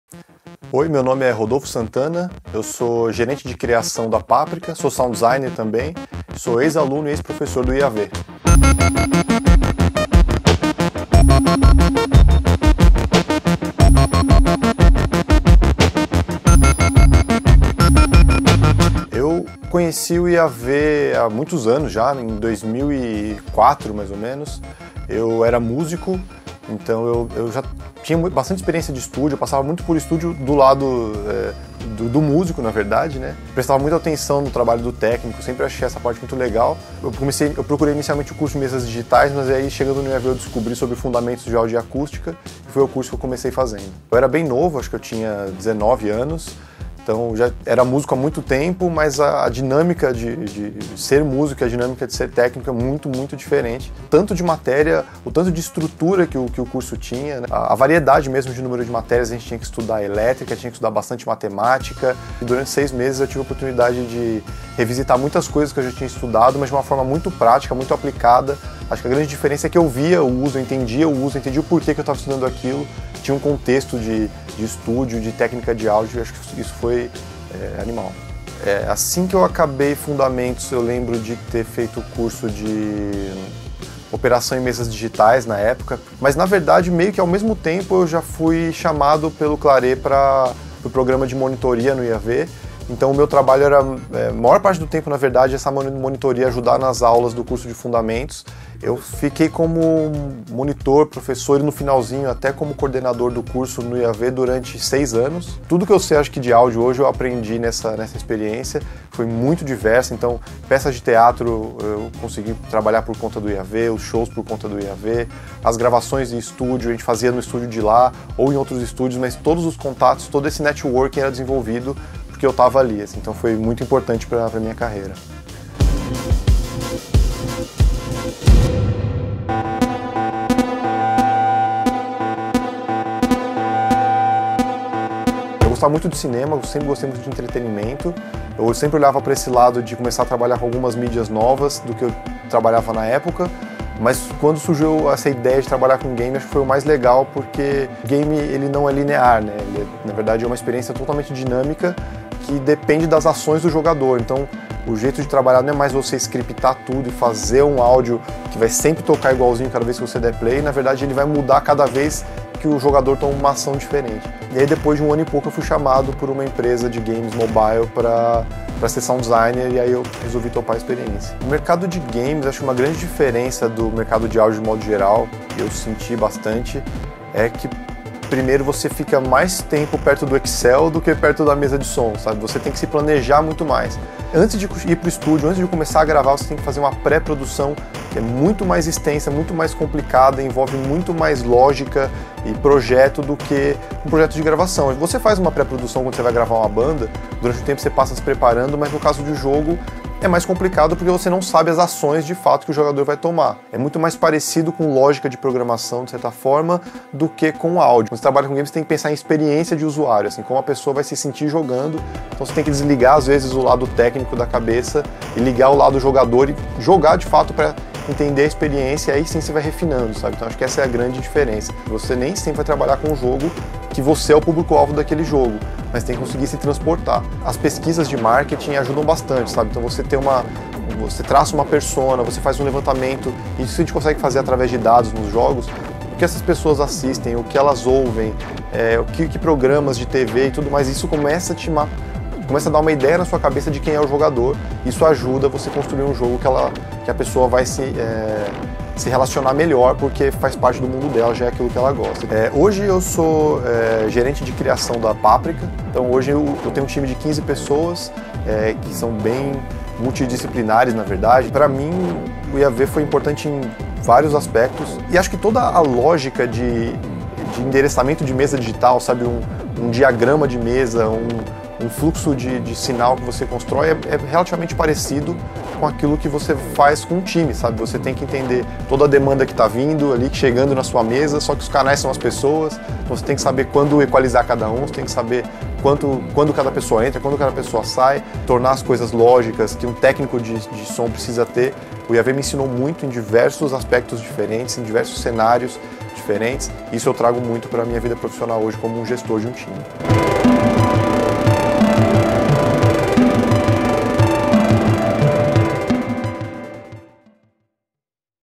Ex-Alunos IAV - Rodolfo Santana
Rodolfo Santana, conhecido como Castor entre os colegas, é Gerente de Criação da Paprika, braço da Tapps Games dedicada a criar soluções de áudio para Jogos Eletrônicos e outras aplicações disruptivas como Realidade Virtual e afins. Além de ter começado sua carreira no áudio no IAV, Castor foi professor e coordenador de curso na escola. Confira o depoimento dele sobre a importância da instituição em sua carreira e como é trabalhar com áudio para games.